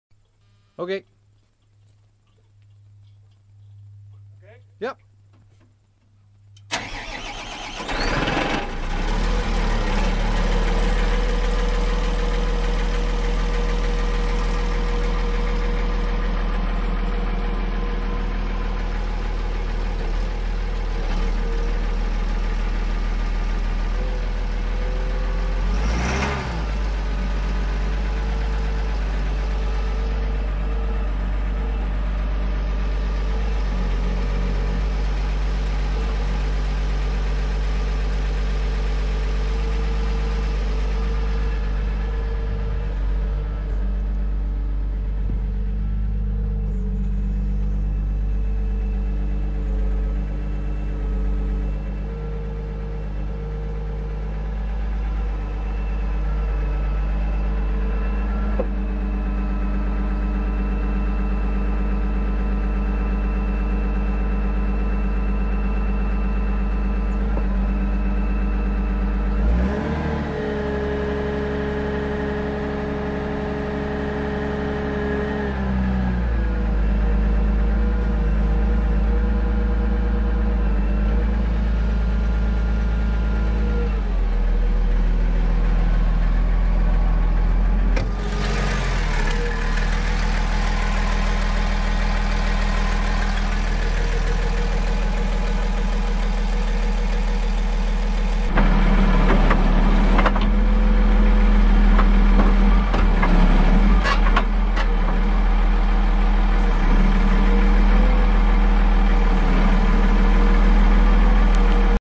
GovDeals: Volvo G960 Grader
Volvo G960 Grader sold by North Glengarry Township, ON on GovDeals.com